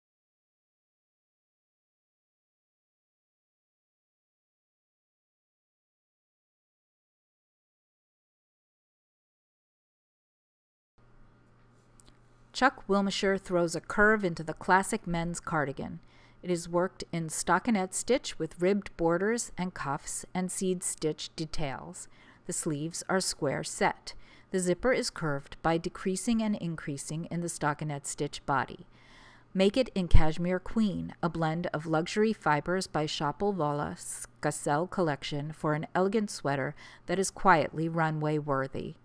#26 Men's Curved Zipper Cardigan, Vogue Knitting Winter 2012/13
Designed by Chuck Wilmesher in Cashmere Queen by Schoppel Wolle/Skacel Collection.